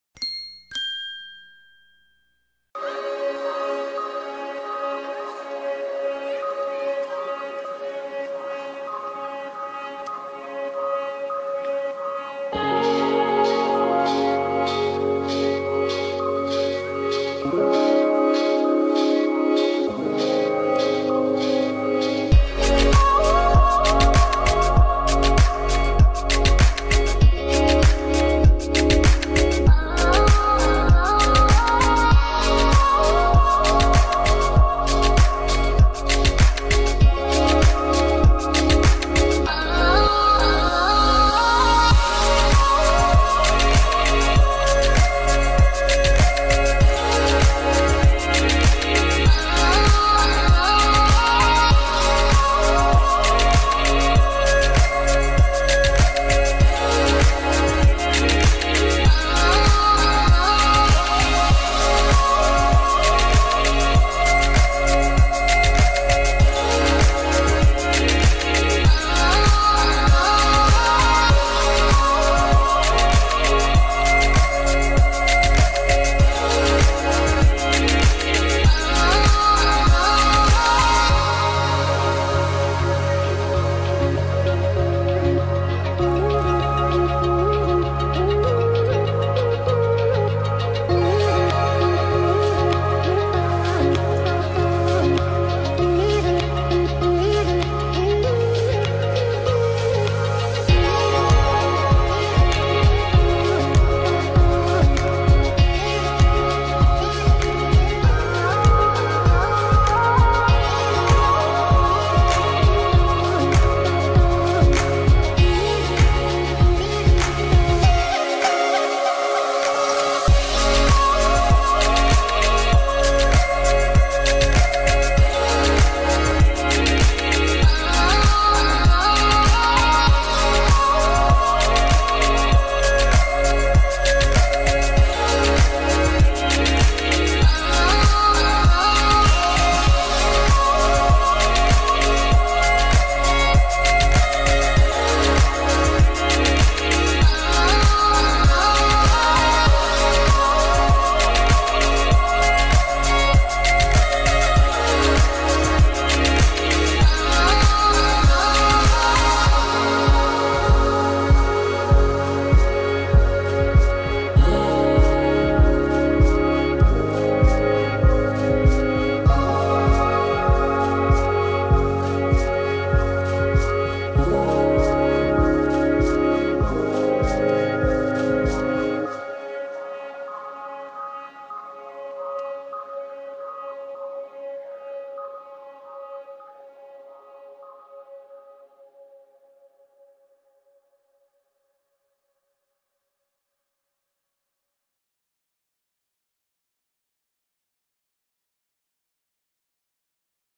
Modelling a simple chair in blender 2.8
In this video tutorial I will show you how to modelling simple chair for beginners